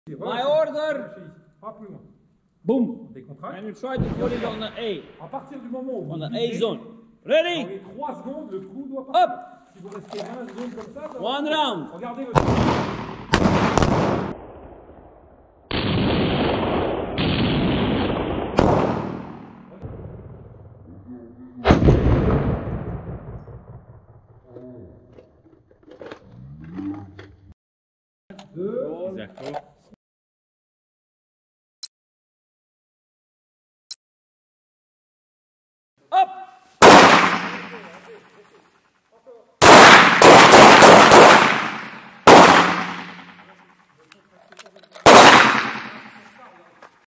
I'm still speechless from this. 😆
My very 1st shot of the day at the Dynamic Pistol Course from ESA Benelux given by Romulus Mihu.